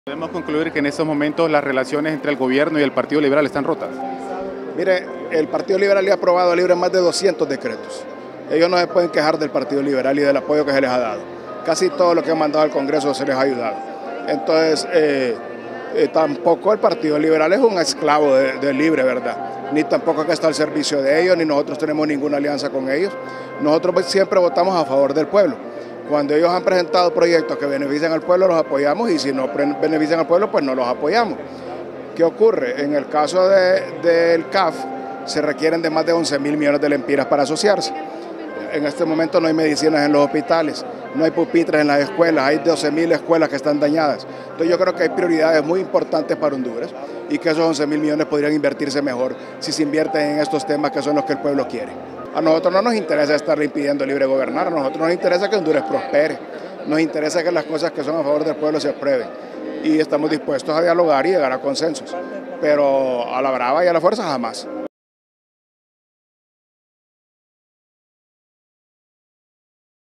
Yani Rosenthal: El Partido Liberal no es “esclavo” de Libre para aprobarle todos los decretos
El Partido Libera le ha aprobado a Libertad y Refundación (Libre), más de 200 decretos, “no se pueden quejar del apoyo que se les ha dado”, tampoco el PL es un “esclavo”, ni está al servicio, ni tenemos una alianza con ellos, “nosotros siempre votamos a favor del pueblo”, refirió el presidente del Consejo Central Ejecutivo del Partido Liberal (CCEPL), Yani Rosenthal.

“Estamos dispuestos a dialogar y llegar a consensos, pero a la brava y la fuerza jamás”, subrayó.